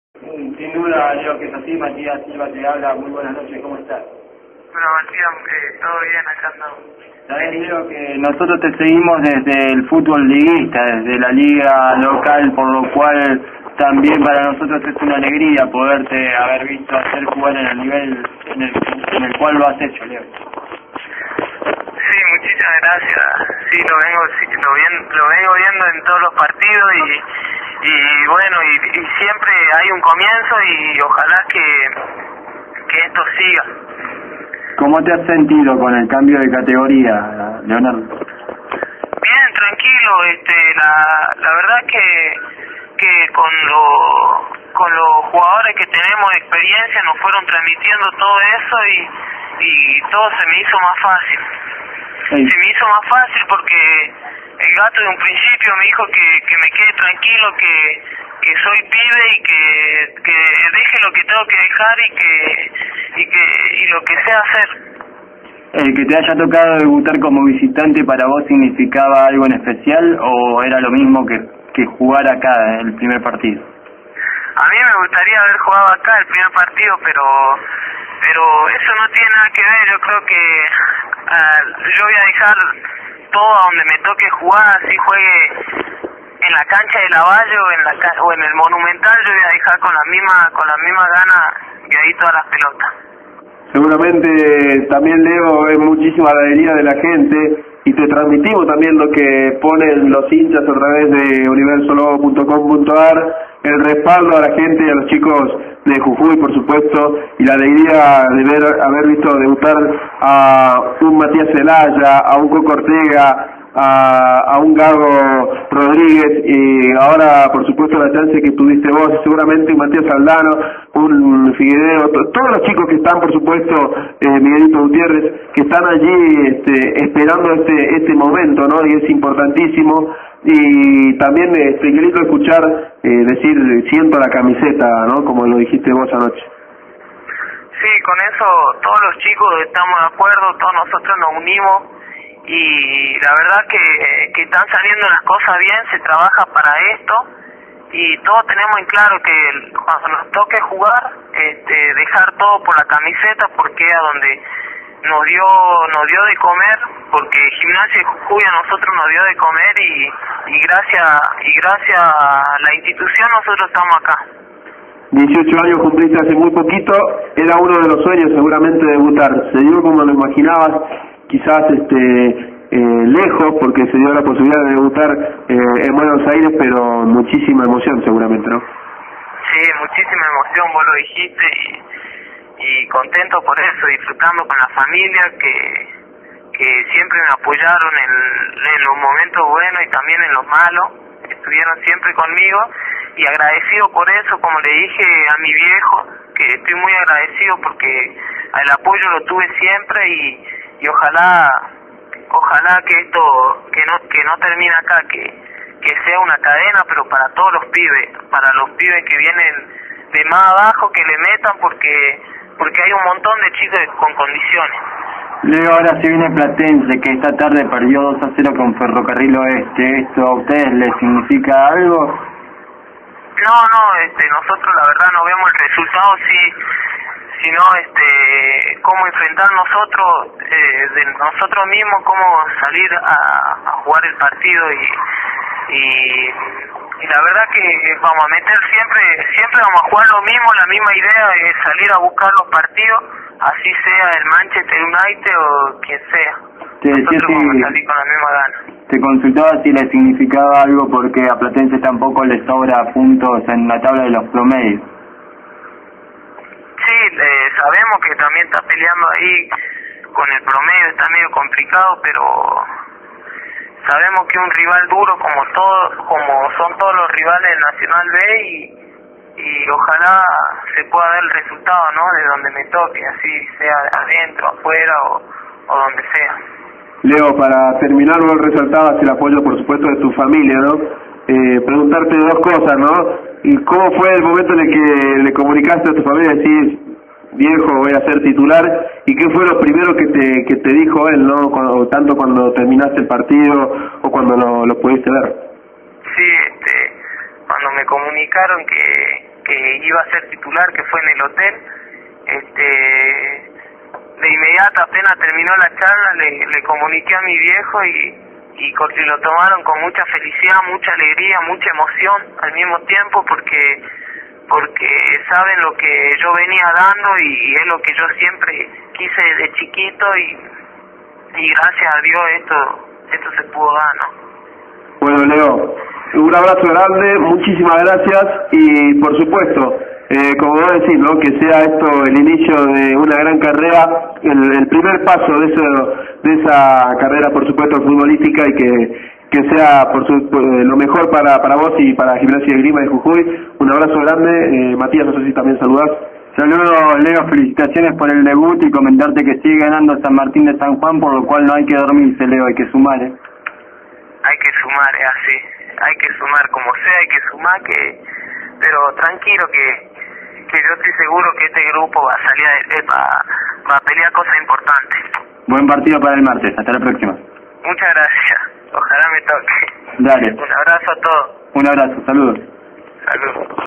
LEO FERREYRA DESPUES DEL DEBUT
El pibe hablo con Radio Noa y Universo Lobo despues del debut.